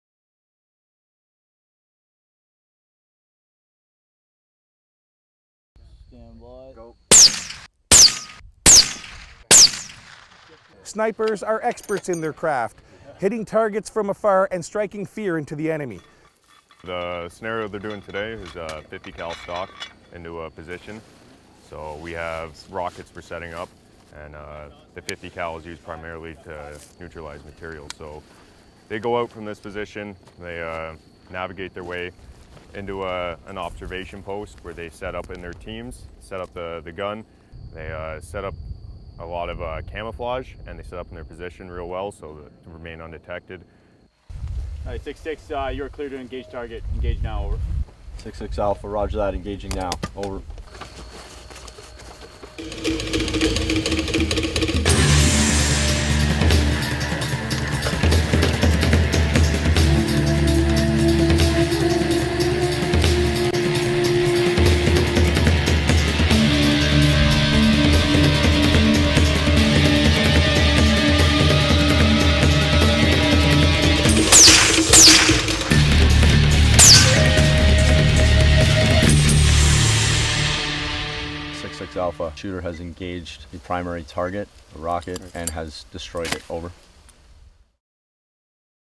Basic Sniper Course
Sgt Marco Comisso recently visited with a Basic Sniper Course conducted at CFB Petawawa. Come behind the scenes with these amazing professionals!